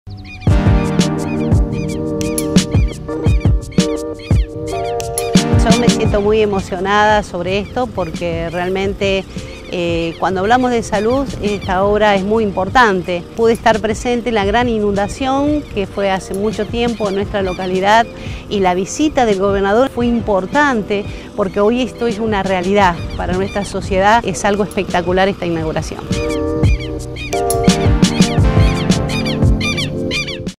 Vicuña Mackena tendrá su nueva planta depuradora de líquidos cloacales
Inauguramos la obra de reacondicionamiento y ampliación de la planta depuradora de líquidos cloacales de Vicuña Mackenna.  A través de esta obra que mejora la calidad de vida, estamos dando respuesta a una necesidad fundamental para los habitantes del sur provincial.